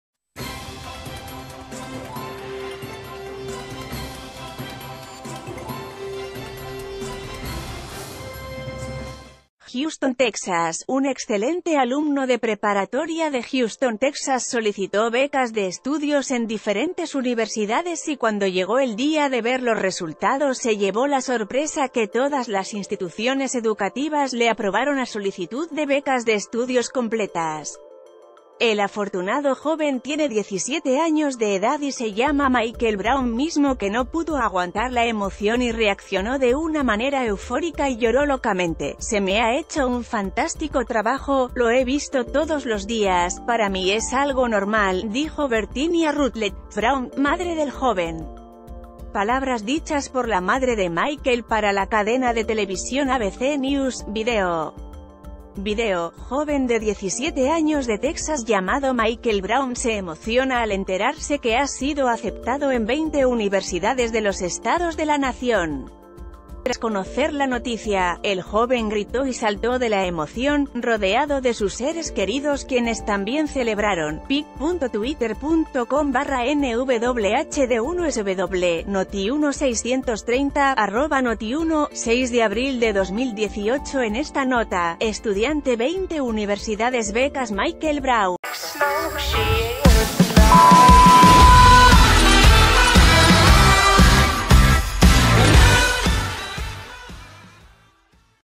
Reacciona eufóricamente al enterarse que es becado en 20 universidades
Houston, Texas.- Un excelente alumno de preparatoria de Houston, Texas solicitó becas de estudios en diferentes universidades y cuando llegó el día de ver los resultados se llevó la sorpresa que todas las instituciones educativas le aprobaron a solicitud de becas de estudios completas. El afortunado...